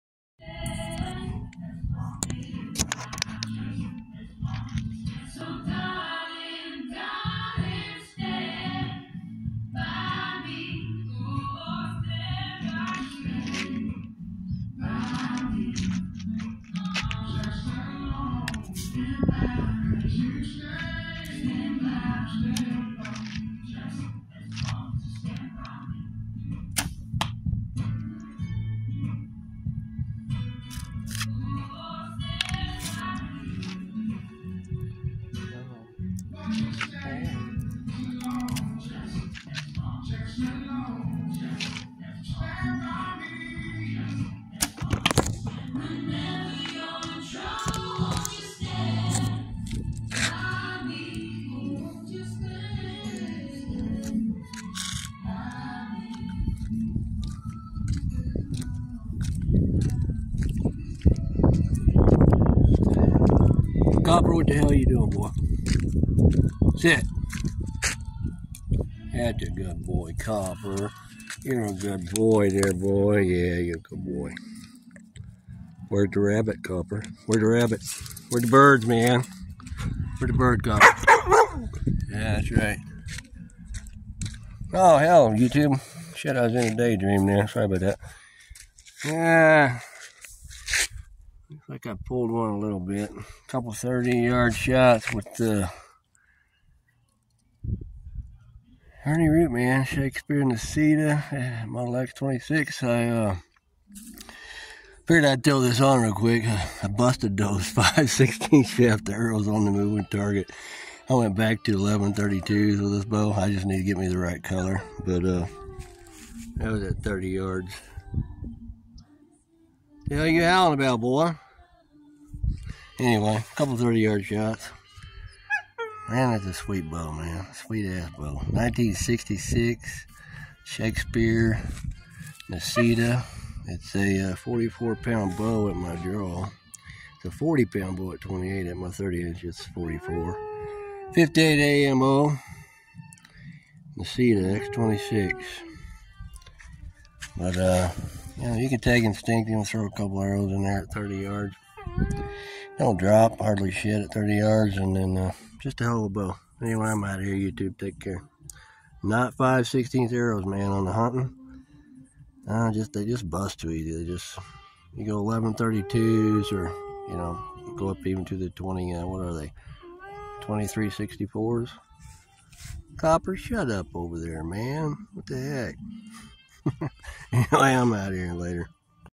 1966 trad bow 11/32 wood couple 30 yard shots, pulled 0ne.
sweet shooter,, smooth draw no none hand shock but still pretty snappy,,,, up next try a couple moving targets see how wood 11/32 hold up